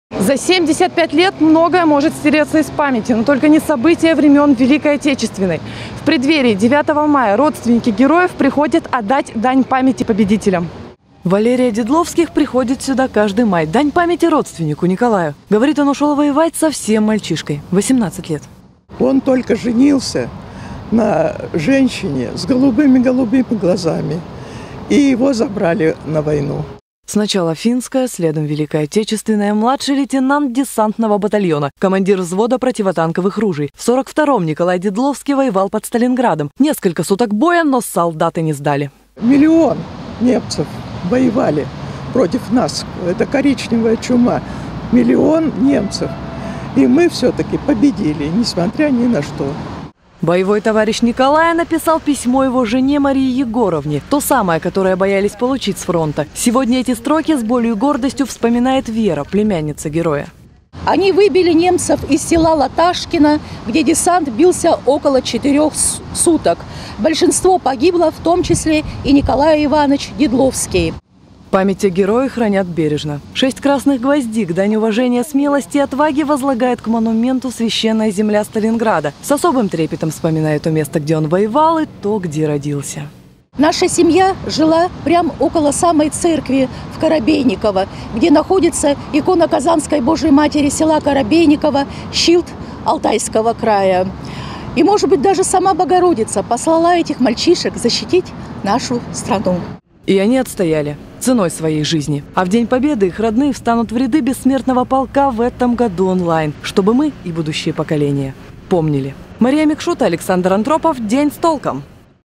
В преддверии Дня Победы барнаульцы вспоминают подвиги героев войны
В связи с пандемией коронавируса - массовые мероприятия в честь Дня Победы отменили. Но несмотря на эпидемиологическую ситуацию в крае, в преддверии памятной даты родственники героев уже начали возлагать цветы на Площади Победы.